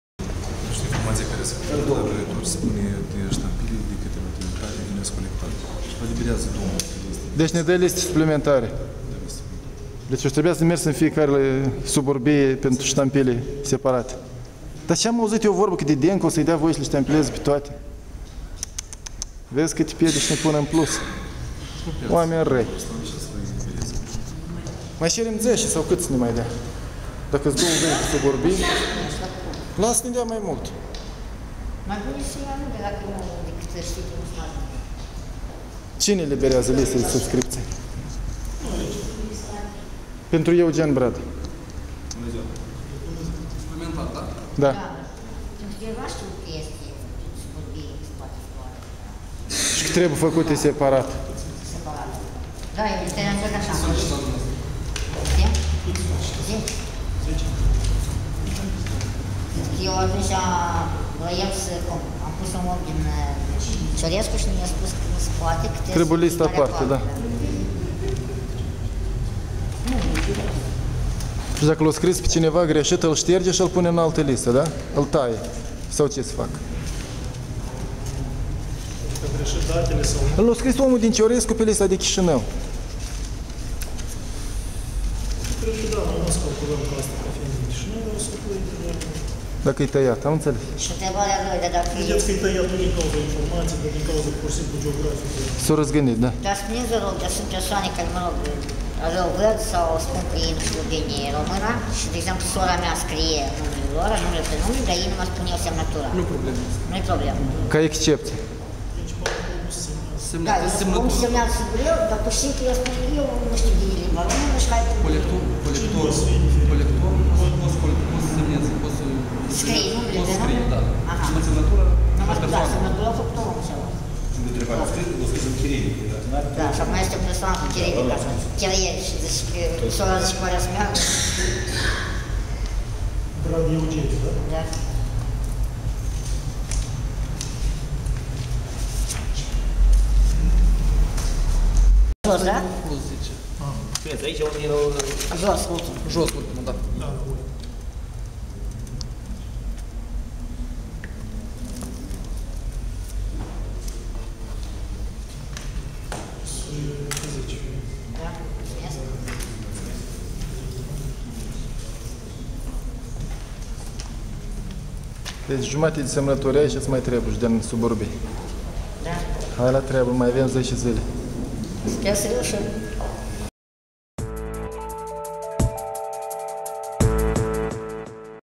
Eugeniu Brad a mai cerut liste de subscripție
Consiliul electoral de circumscripție i-a eliberat doar 10-11 liste, dar numai suburbii în Chișinău sunt de două ori mai multe, și pentru fiecare localitate trebuie listă aparte. Azi Eugen a venit cu tatăl său-însoțitor la CEC să ceară mai multe liste. Deja a colectat jumătate din numărul necesar de semnături din oraș, acum s-ar putea îndrepta și spre suburbii ca să facă plinul.
Președintele DEBRA-Moldova intenționează să candideze independent la un loc de consilier municipal.